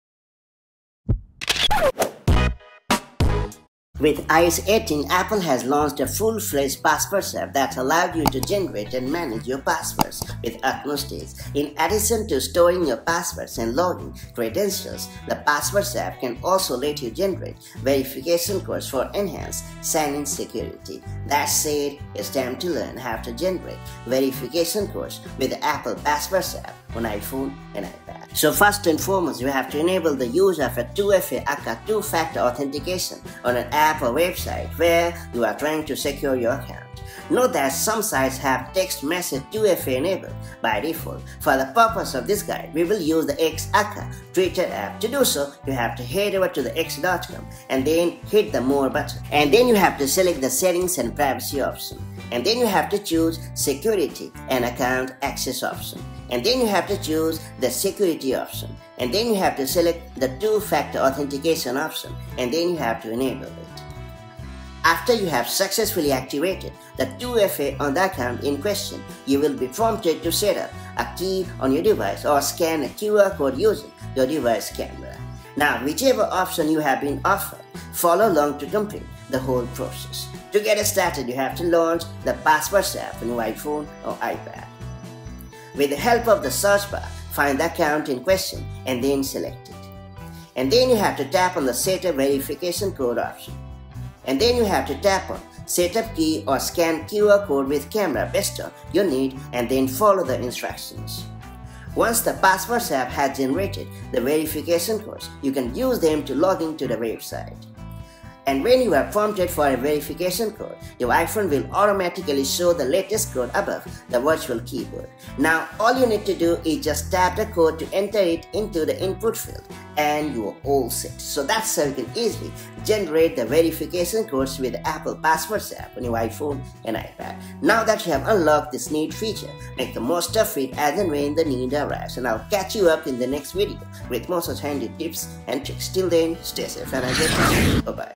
How to Generate Verification Codes With Passwords App in iOS 18 on iPhone/iPad
Wondering how to generate the verification codes for enhanced sign-in security using the Apple Passwords app in iOS 18/iPadOS 18 on your iPhone and iPad? Check out this detailed guide to find out: